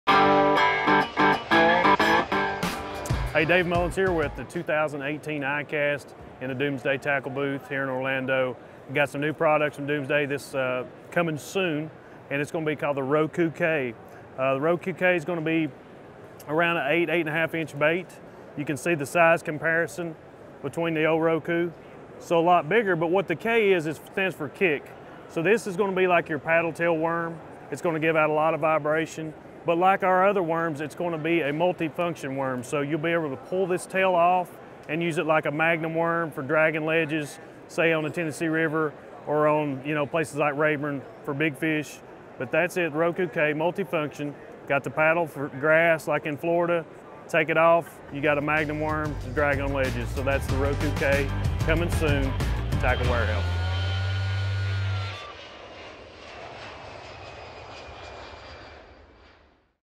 Doomsday Roku K Cut Tail Worm with David Mullins | iCast 2018
TW discusses the Doomsday Roku K Cut Tail Worm with David Mullins at the 2018 ICAST Show in Orlando, Florida.